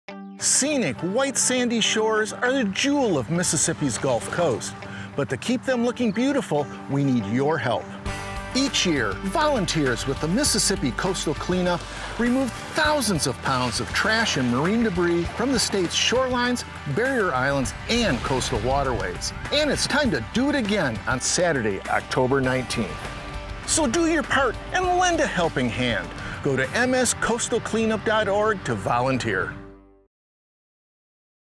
2019 Coastal Cleanup PSA YouTube
Scenic, white sandy shores are the jewel of Mississippi’s Gulf Coast…. But to keep them looking beautiful), we need your help! Each year, volunteers with the Mississippi Coastal Cleanup, remove thousands of pounds of trash and marine debris from the state’s shorelines, barrier islands and coastal waterways. And it’s time to do it again on Saturday October 19th! So do your part and lend a helping hand! Go to mscoastalcleanup.org to volunteer.